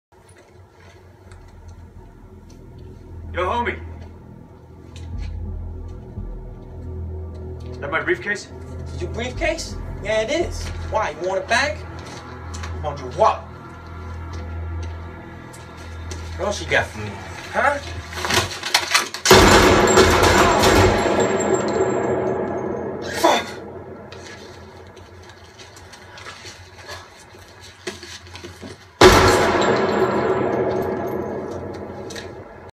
Some of the Best Gun Shots Put to Film.
More test scenes on my home theatre system